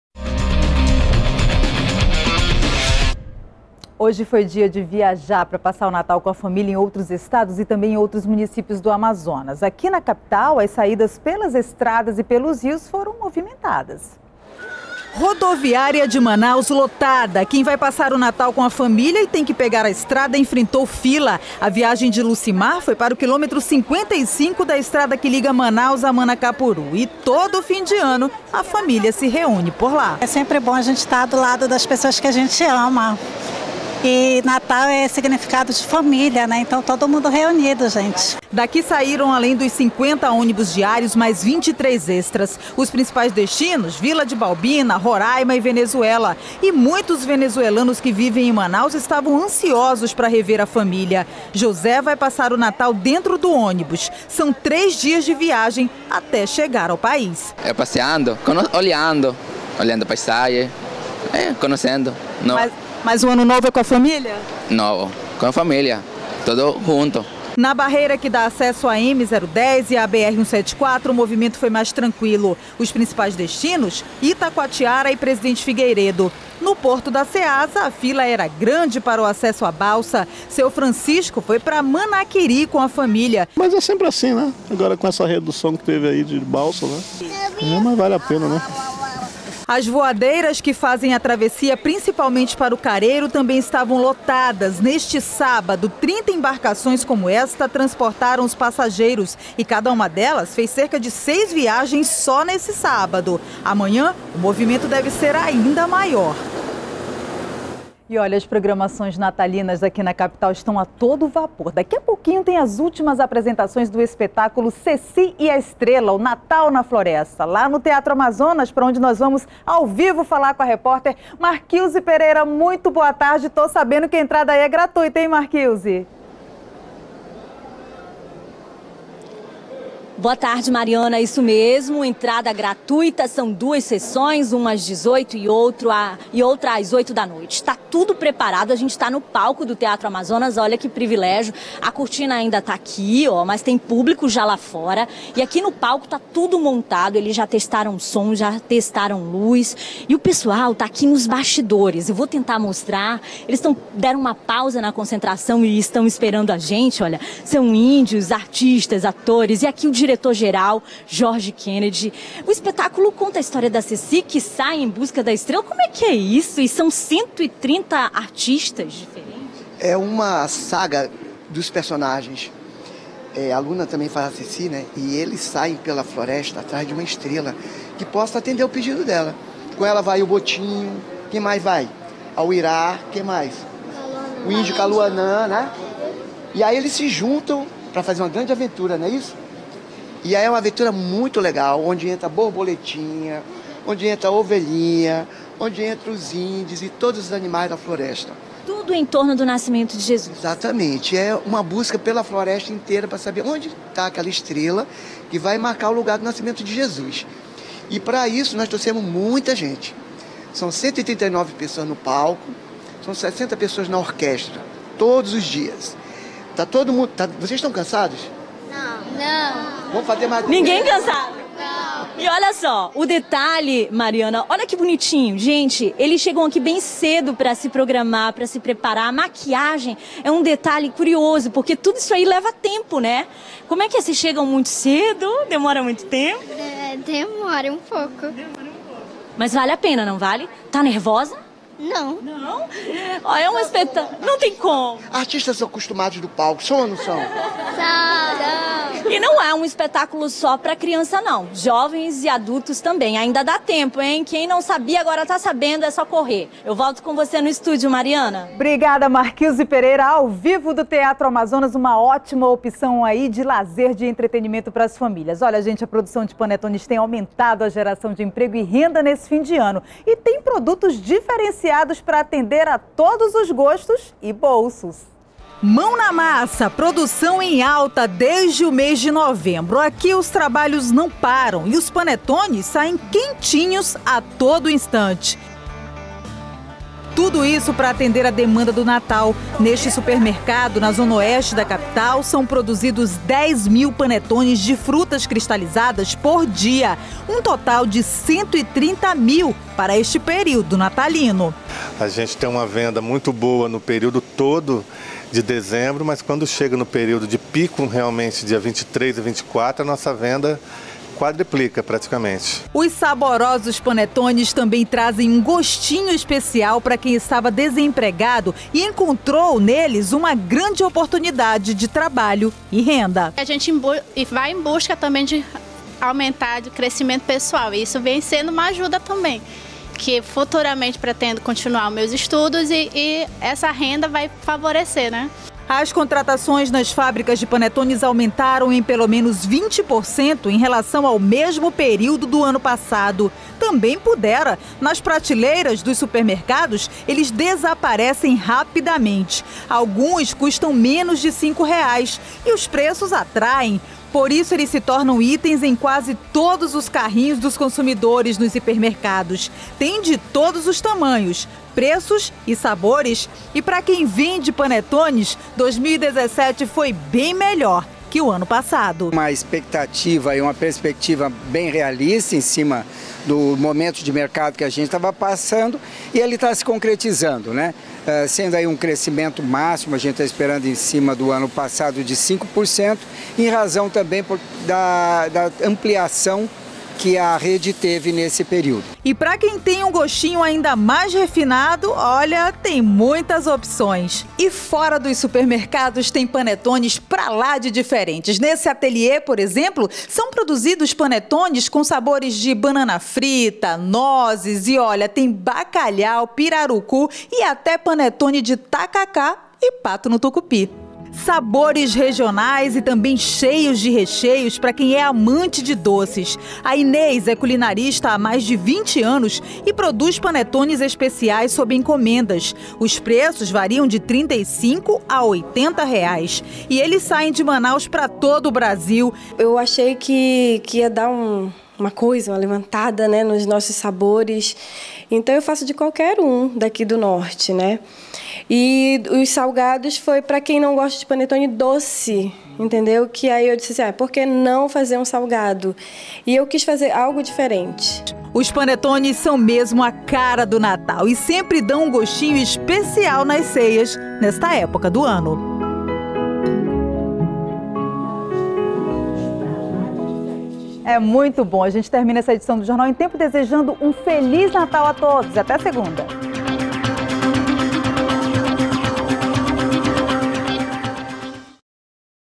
Conferira como é feita a produção de Panetones tradicionais e especiais
03º BLOCO JORNAL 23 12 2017 - Conferira como é feita a produção de Panetones tradicionais e especiais